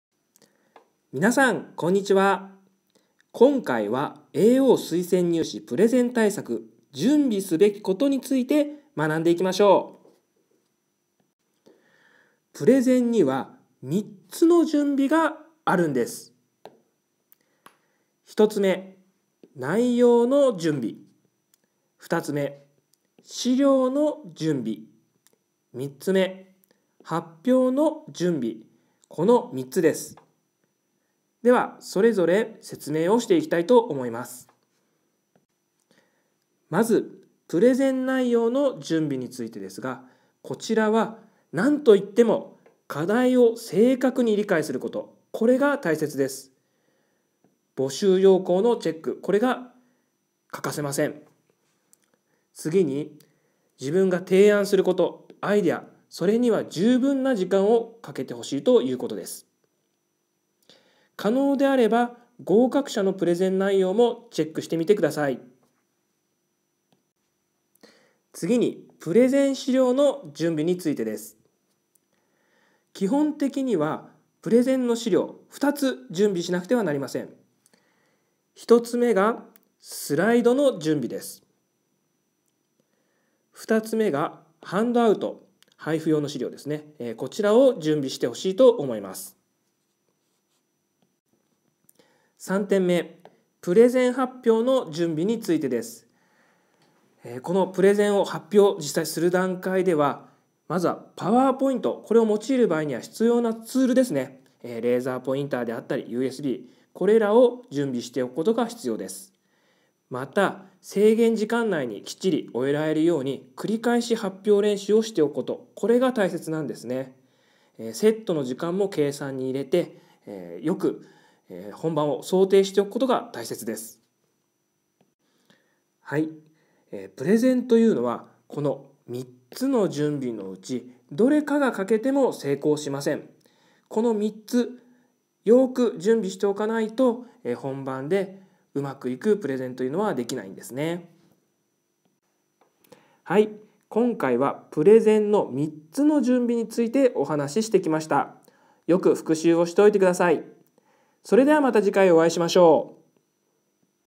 ゼロからわかる！合格するAO推薦入試 プレゼン対策 無料講座②準備すべきこと
【プレゼン対策でお悩みのAO・推薦入試受験生 必見！】
合格するAO推薦入試 大学プレゼンの無料対策講座です。ゼロから丁寧にプレゼン（準備すべきこと）について解説していきます。
AO・推薦入試専門塾 KOSSUN教育ラボでは、国公立大、私立大問わず、プレゼン対策に完全対応しています。